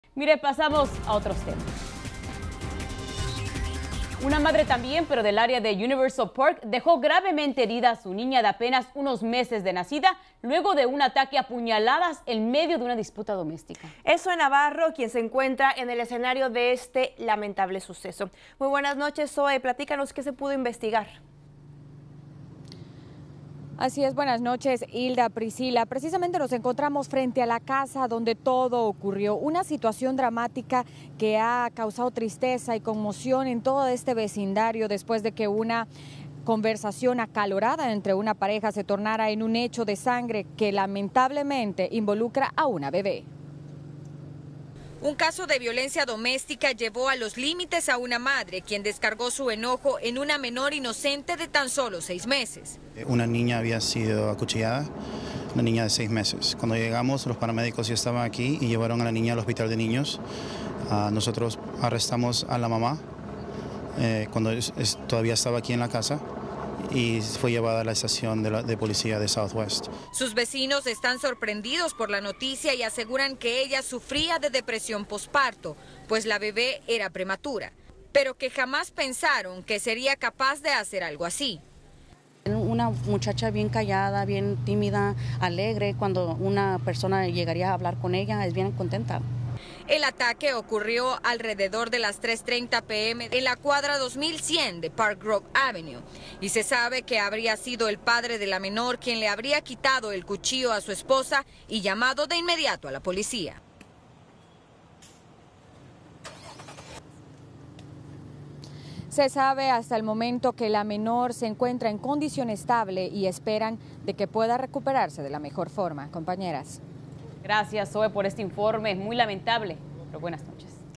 Madre apuñala a su bebé de seis meses - Noticias 62
Una madre del área de University Park dejo gravemente herida a su niña de apenas unos meses de nacida, luego de un ataque a puñaladas en medio de una disputa doméstica.